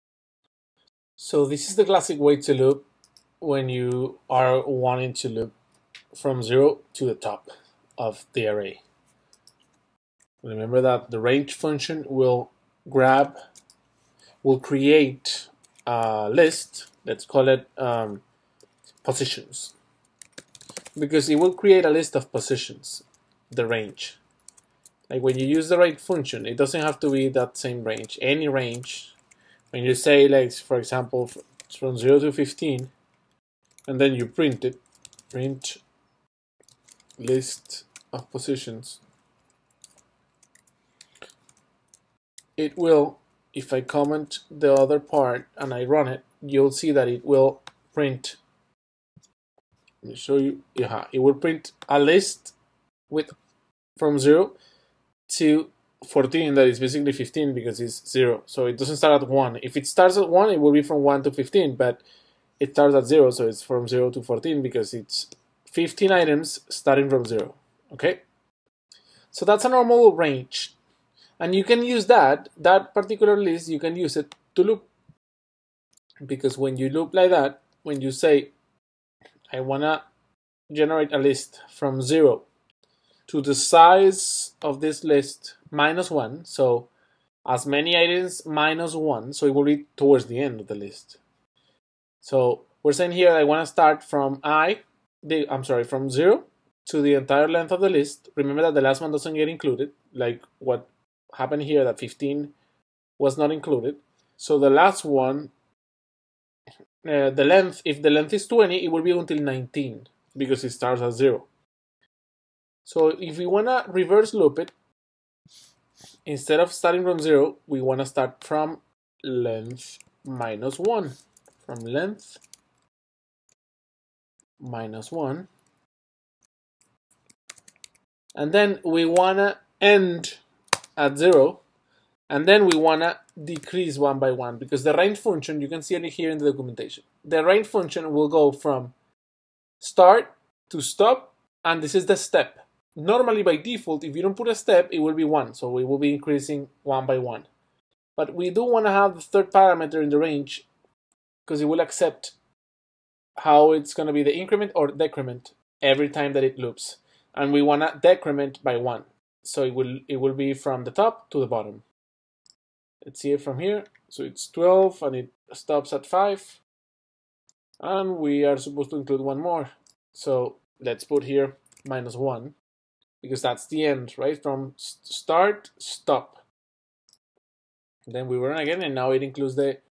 02.1 Reverse looping array in python - Python List Exercises Tutorial Interactive
You can also reverse loop an array because the range function allows to increase or decrease each iteration.

This video is a part of the Python Interactive List exercise tutorial, you can access the entire tutorial here: